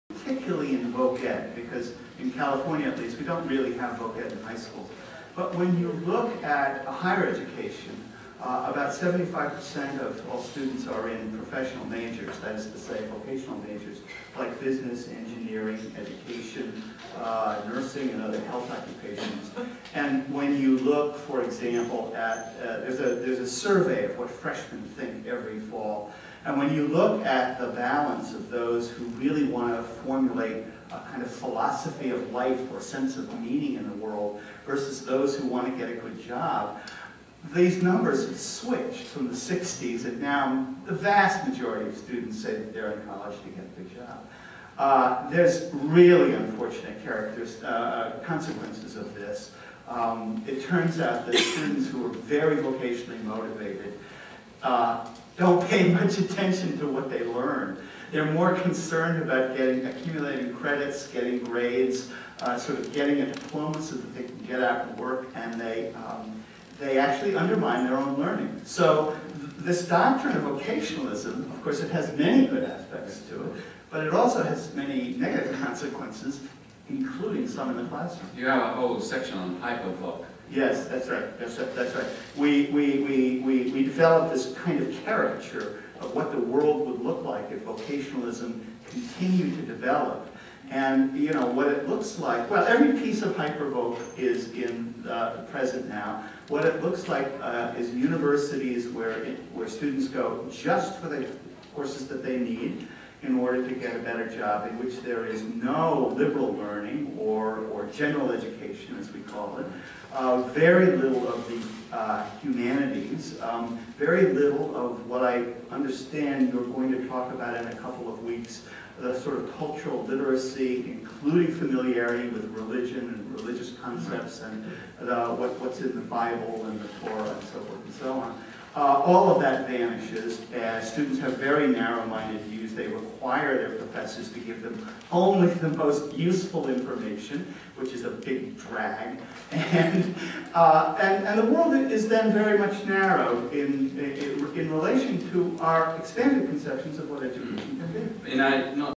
Norton Grubb
Examining the state of american education Dr Norton Grubb of the University of California Berkeley will discuss what parents should now abouth the systems in which their children are raised and nurtured.